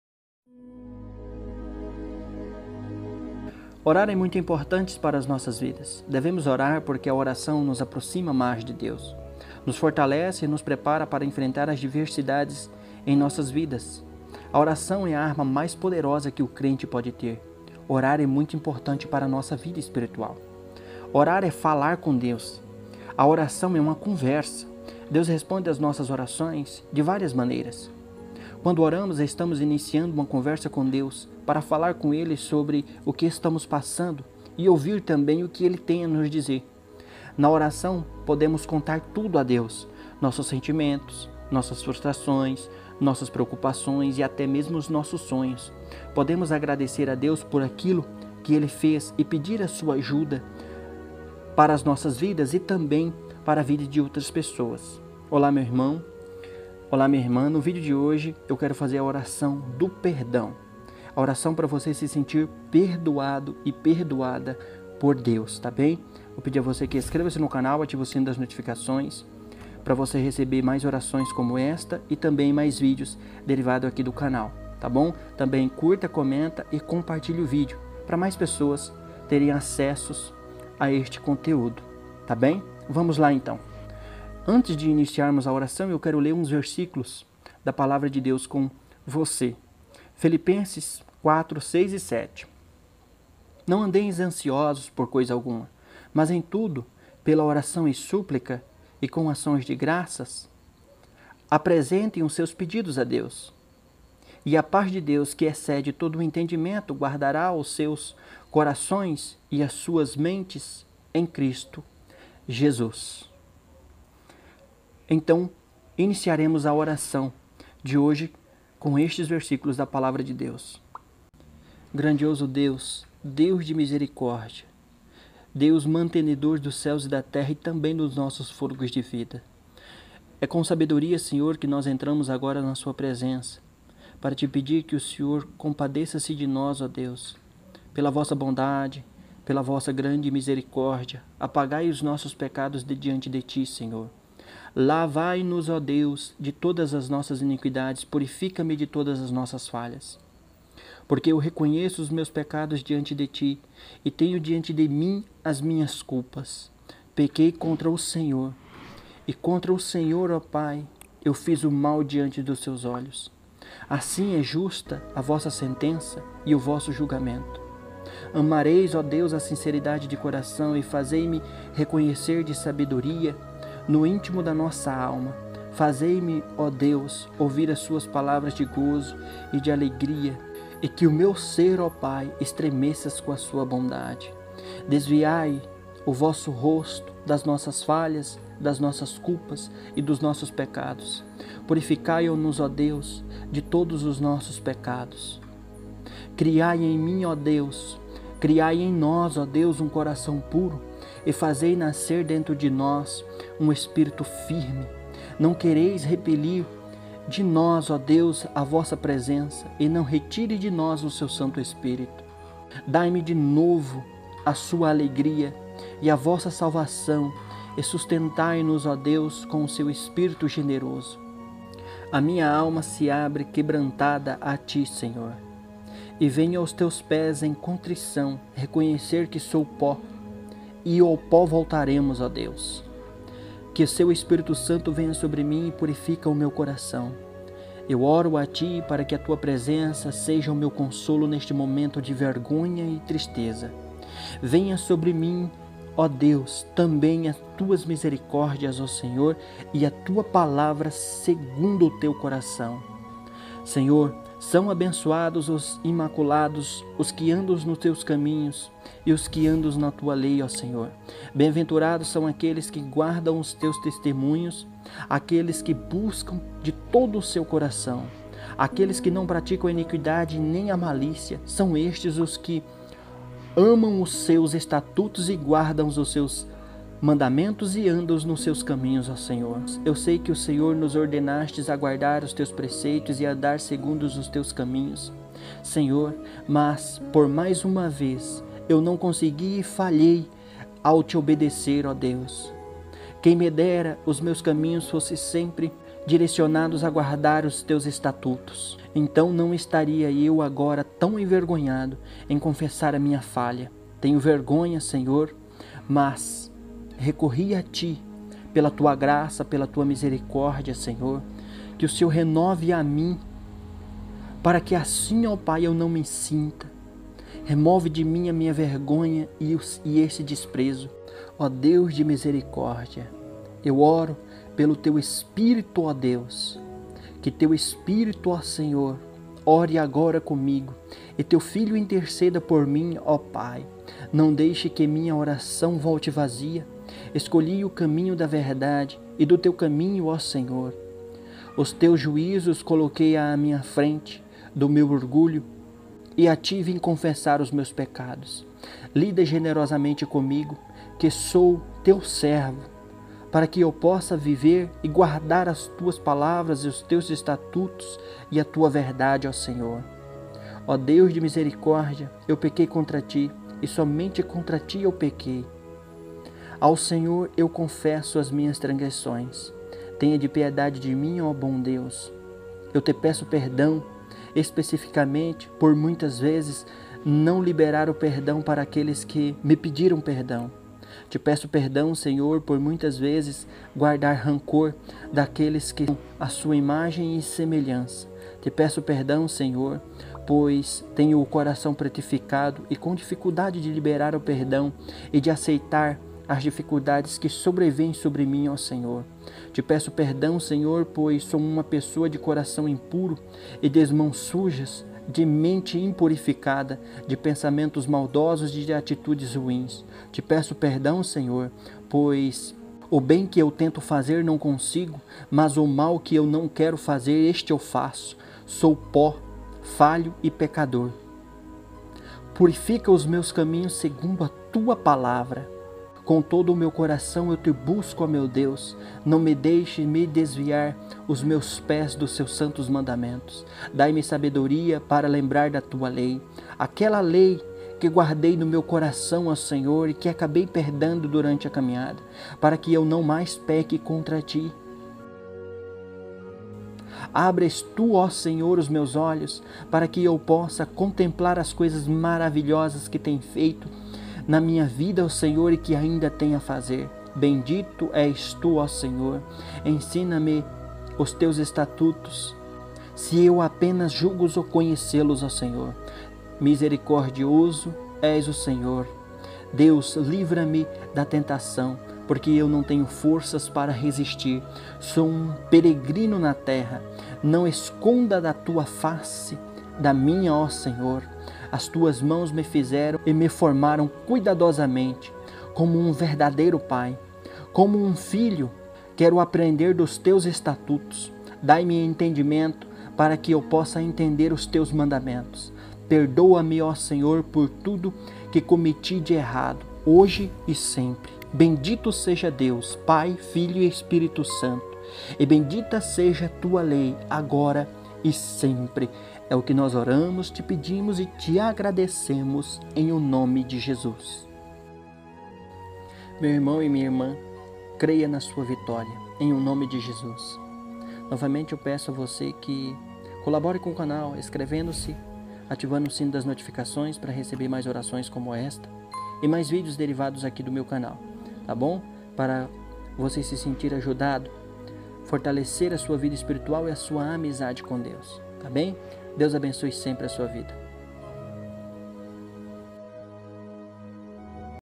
ORAÇÃO PARA DEUS TE PERDOAR, ORAÇÃO DO PERDÃO | Wellinton Jesus
Por que devemos orar?
Oração para sentir o perdão de Deus.

Redes Social.

Devemos orar porque a oração nos aproxima de Deus, nos fortalece e nos prepara para enfrentar as dificuldades da vida. A oração é a arma mais poderosa do crente. Orar é muito importante para a vida espiritual.
Orar é falar com Deus. A oração é uma conversa. Deus responde às nossas orações de várias maneiras. Quando oramos, estamos iniciando uma conversa com Deus, para falar com Ele sobre o que está passando e ouvir o que Ele tem a dizer.

Na oração podemos contar tudo a Deus: nossos sentimentos, nossas frustrações, nossas preocupações, nossos sonhos... Podemos agradecer a Deus por aquilo que fez e pedir Sua ajuda para nossas vidas e as vidas de outras pessoas (Filipenses 4:6-7).
"Não andem ansiosos por coisa alguma, mas em tudo, pela oração e súplicas, e com ação de graças, apresentem seus pedidos a Deus.
E a paz de Deus, que excede todo o entendimento, guardará os seus corações e as suas mentes em Cristo Jesus."

MUITO OBRIGADO NOVAMENTE PELA VISITA, VOLTE SEMPRE.